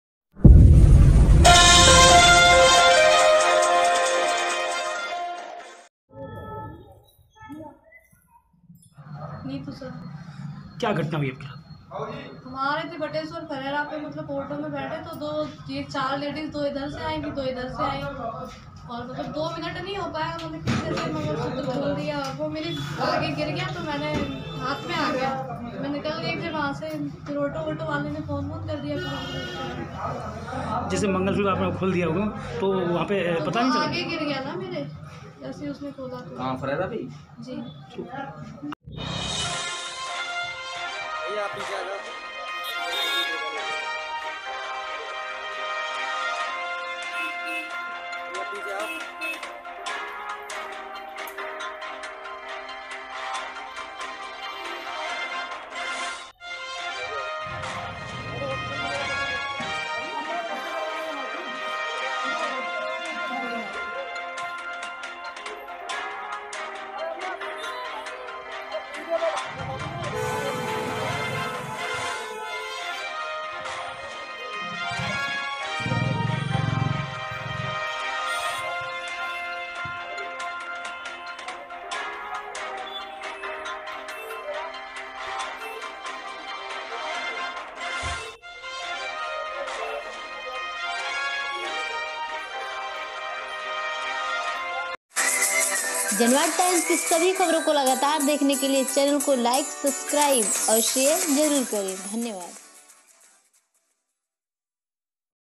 Agra UP | पिनाहट पुलिस ने लुटेरी महिलाओं का गैंग पकड़ा, 4 महिलाएं गिरफ्तार
जनवाद टाइम्स : Agra UP | पिनाहट पुलिस ने लुटेरी महिलाओं का गैंग पकड़ा, 4 महिलाएं गिरफ्तार

पिनाहट पुलिस ने लुटेरी महिलाओं का गैंग पकड़ा। महिला ऑटो में सवार होकर अपने मायके से ससुराल जा रही थी। लुटेरी महिलाओं के गैंग ने एक महिला का मंगलसूत्र तोड़कर इको गाड़ी में सवार होकर आगरा की ओर भाग रहा था। पुलिस ग्रामीणों की सूचना पर घेराबंदी कर महिलाओं के गैंग को इको गाड़ी ड्राइवर सहित दबोच लिया।

जानकारी के अनुसार नीतू शर्मा पत्नी सोनू शर्मा निवासी बिजलीपुरा थाना महुआसाला बुधवार को दोपहर करीब 3 बजे गांव बटेश्वर अपने मायके से ऑटो में बैठ कर भदरौली की ओर आ रही थी। तभी फरेरा चौराहे पर पहले से इको गाड़ी में महिलाओं का गैंग महिला ताक रहा था। इको गाड़ी से उतरकर महिलाओं गैंग ऑटो में आ गया महिला को चारों ओर से घेरकर महिला के गले से मंगलसूत्र को काट दिया। महिला का मंगलसूत्र जमीन पर गिर गया। महिला ने चिल्लाना शुरू कर दिया महिला की आवाज सुनकर आसपास के लोग एकत्रित हो गए भीड़ को देखकर महिलाओं का गैंग इको में सवार होकर आगरा की ओर भागने लगा। ग्रामीणों ने तत्काल महिला गैंग की सूचना चौकी भदरौली को दी पुलिस ने तत्काल थानाध्यक्ष अमित कुमार को घटना की जानकारी दी। थानाध्यक्ष ने सतर्कता दिखाते हुए पुलिस फोर्स के साथ घेराबंदी की इको गाड़ी में बैठा महिलाओं का गैंग पुलिस को देख कर भागने लगा। पुलिस ने घेराबंदी कर 4 महिलाओं और ड्राइवर को इको गाड़ी सहित गिरफ्तार कर लिया। थानाध्यक्ष अमित कुमार ने महिलाओं की गैंग को पकड़ कर इको गाड़ी सहित थाना बाह पुलिस को सौंप दिया। पुलिस ने मामले की जांच पड़ताल शुरू कर दी है। वही ग्रामीणों ने पिनाहट को पुलिस को धन्यवाद दिया।

बाइट- नीतू शर्मा, पीड़ित महिला

 सिटी टुडे समाचार ,बुलंद भारत बुलंद खबर ,ब्यूरो चीफ नीरज यादव की रिपोर्ट, कैमरामैन अभिषेक यादव के साथ